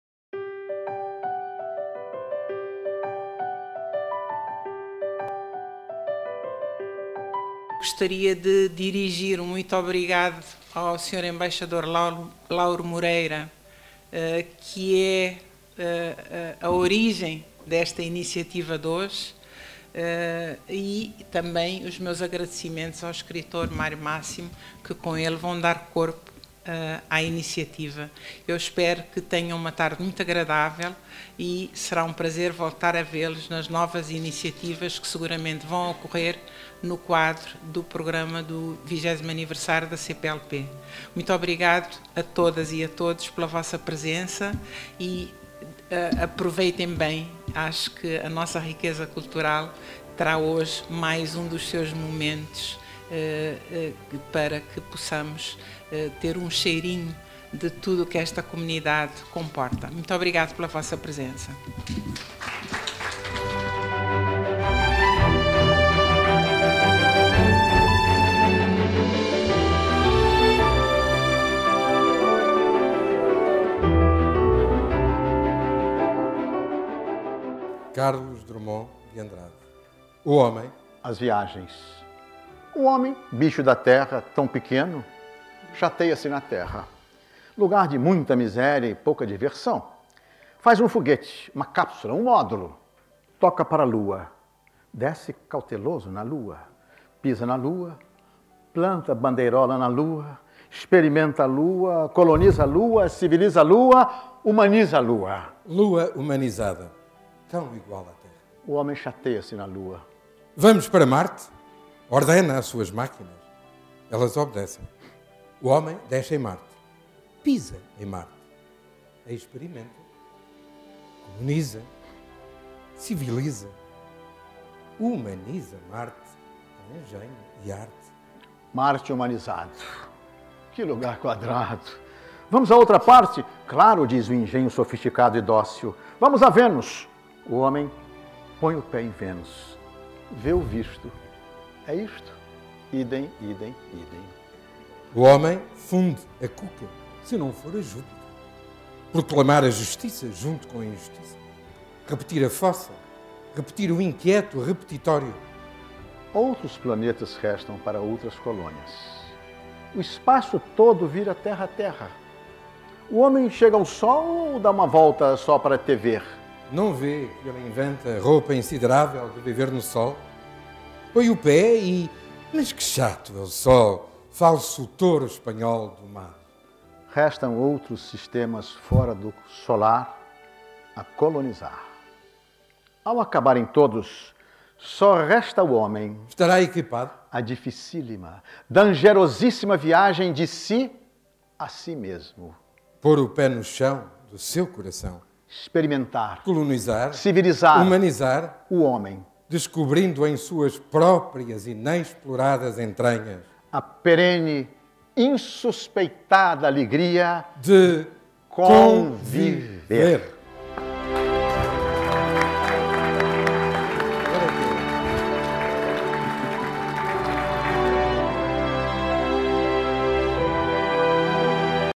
Sarau Cultural: "Vozes Poéticas da Língua Portuguesa"
Discurso de abertura do Sarau Cultural pela Diretora-Geral, Georgina Benrós de Mello, e declamação do poema "O homem; as viagens", de Carlos Drummond de Andrade, nas vozes do embaixador Lauro Moreira e o escritor, Mário Máximo.

A Comunidade dos Países de Língua Portuguesa (CPLP) acolheu no auditório do Palácio Conde de Penafiel, no dia 24 de maio, um Sarau Cultural com dois momentos: “Vozes Poéticas da Língua Portuguesa” e a exposição “Rostos de Timor-Leste”.